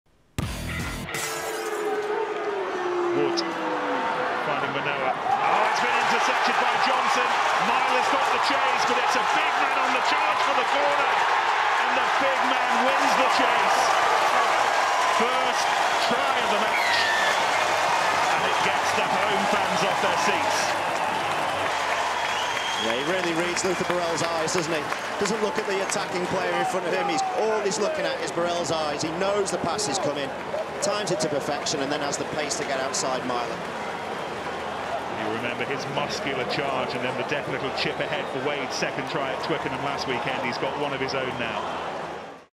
Ashley Johnson's intercept try vs Northampton Saints
Ashley Johnson makes a great interception and scores the first try of the match as Wasps beat the reigning champions Northampton at Adams Park.